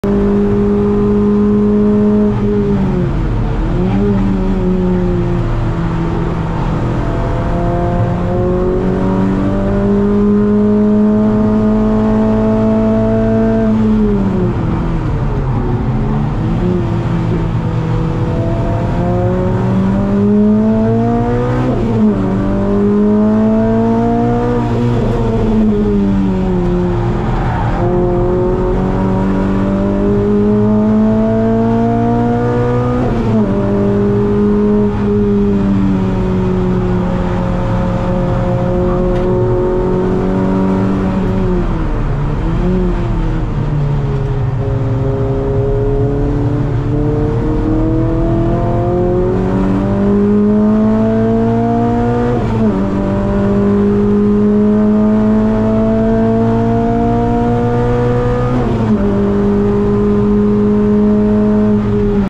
Brands Hatch Clio 200 - Drying/Wet - 59.63s
Constantly changing conditions but had a few laps with a dry line, was good fun trying to find the grip.

PS4 / AST5100 / Gripper